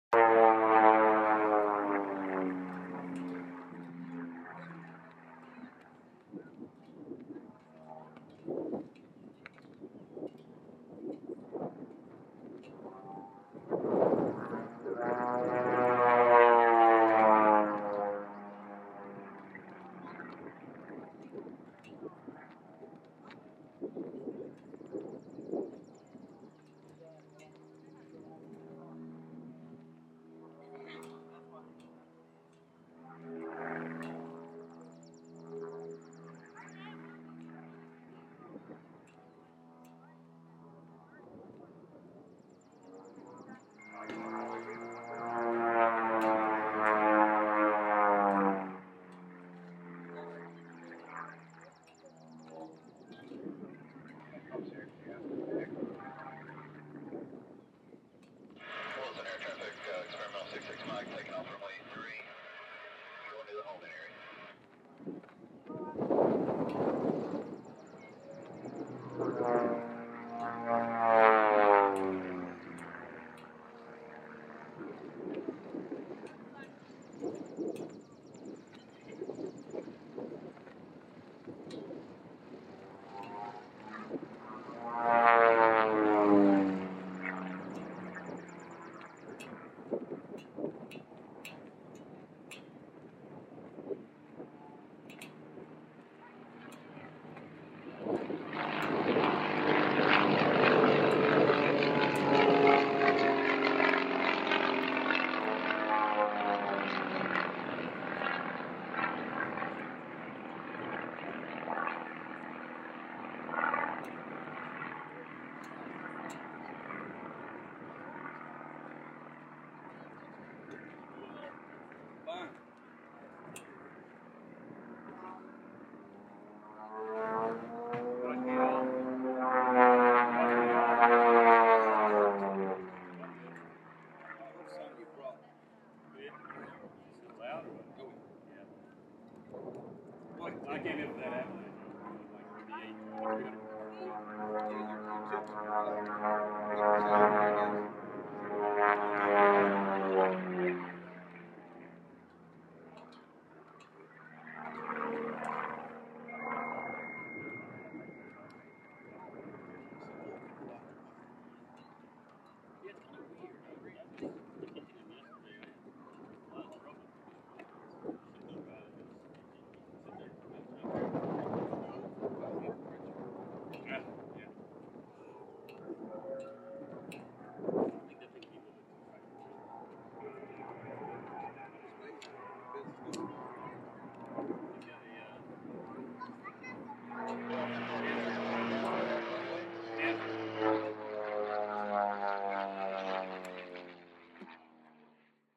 Eric Aero Wilson
Eric Sandifer's Pitts Special performance in the 2012 Carolina Boogie at Wilson NC Industrial Airport, May 18.